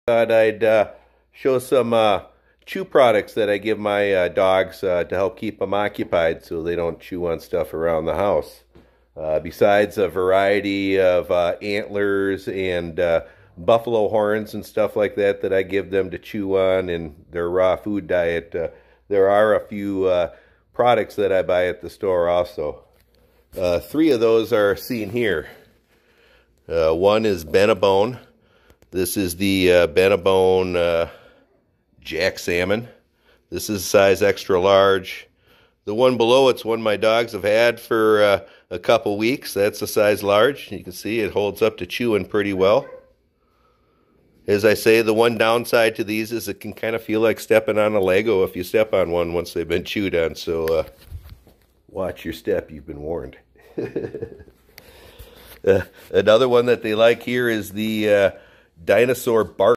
Dog Chew Toys For Extreme Chewers - Benebone
I review brands of extreme dog chew toys my dogs use and love. These products come in multiple sizes and can be found at pet stores, Target, Amazon and multiple other places where pet supplies are sold. These are all products I have bought for my dogs multiple times.

!!! 1st 508th Airborne TEESPRING MERCHANDISE...